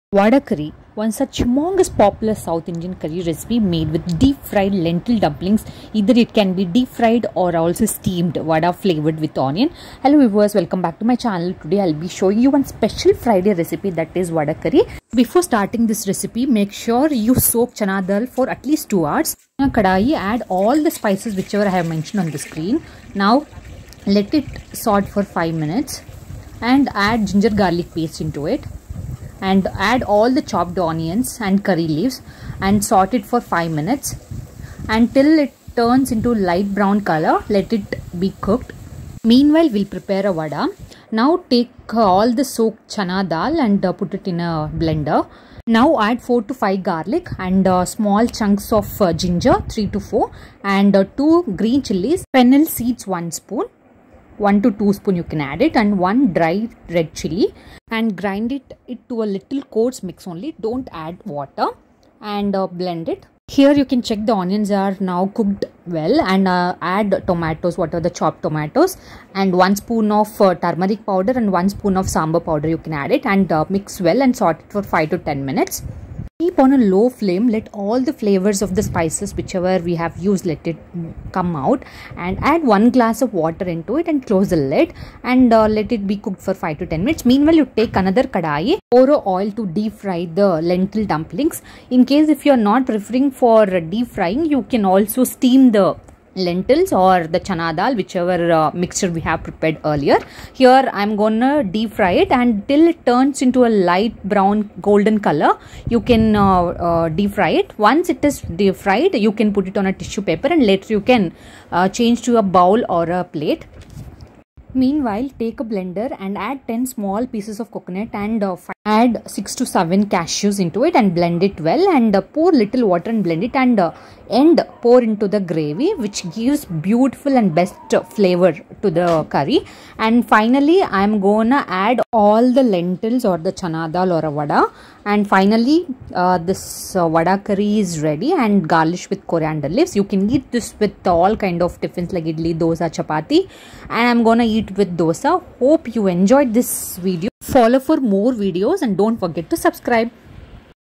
Try this Yummy Vadacurry to your Morning Breakfast!!
Testimony that vada curry originates from Namma Chennai. Vada curry is a simple working class dish of Chennai, North Chennai to be precise.
Traditionally, Vadacurry is a dish made with leftover vadai. Perfect as a side for idli / dosa.